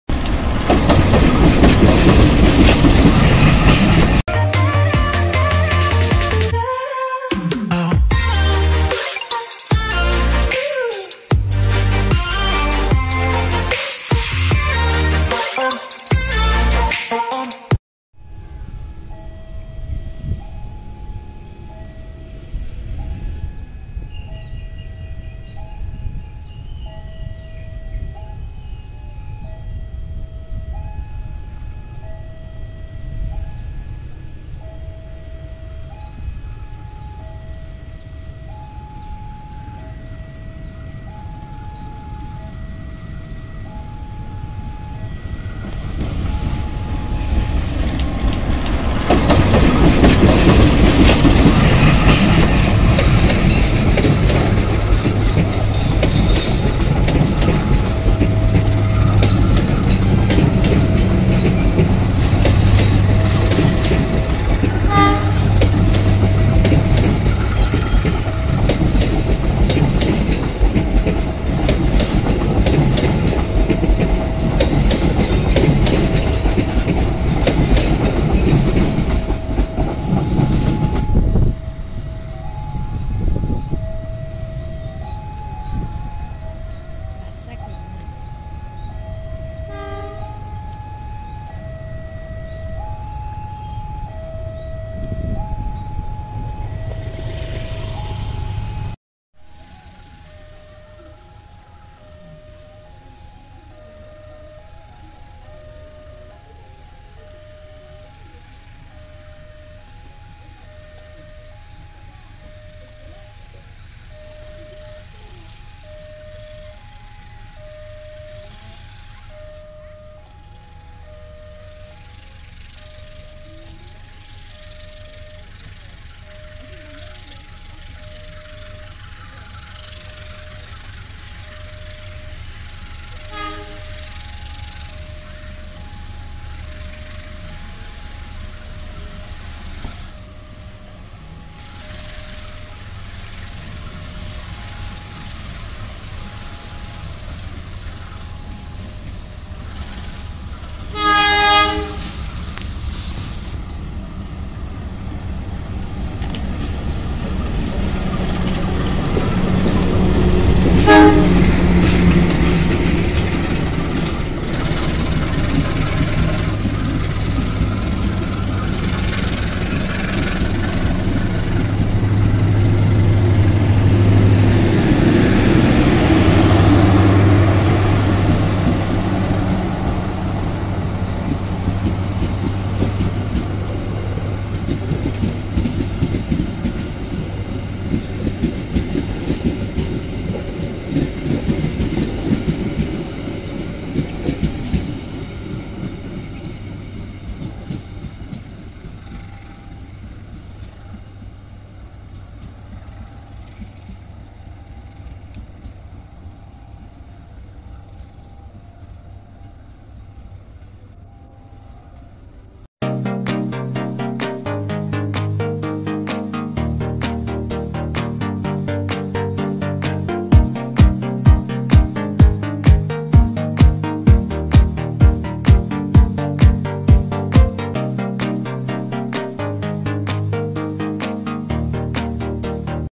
Lokomotif Hidung Panjang Menarik Kereta Api Logawa
Longhood ! Lokomotif Hidung Panjang Kereta Api Logawa

kereta api logawa ditarik lokomotif cc 201 posisi longhood atau hidung panjang